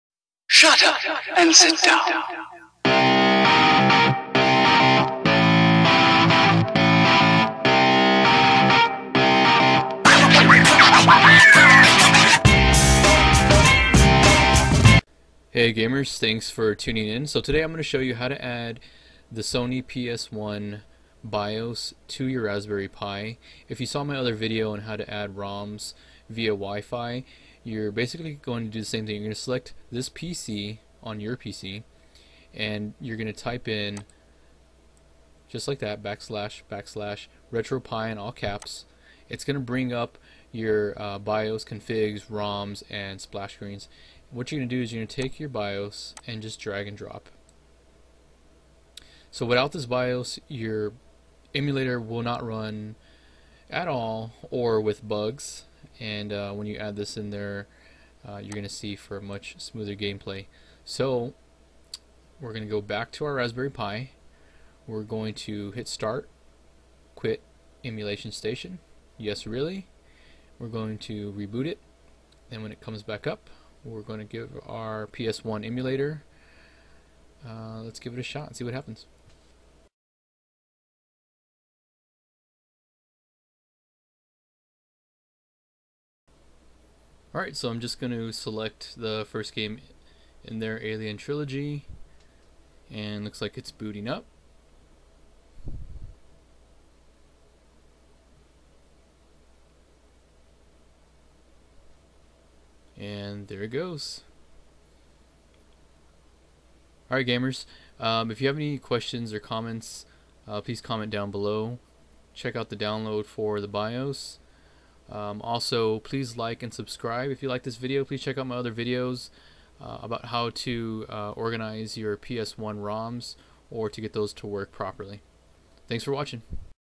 How to install psone bios on Raspberry Pi 3
How to install PS One bios on Raspberry Pi (Retropie) on Episode 4.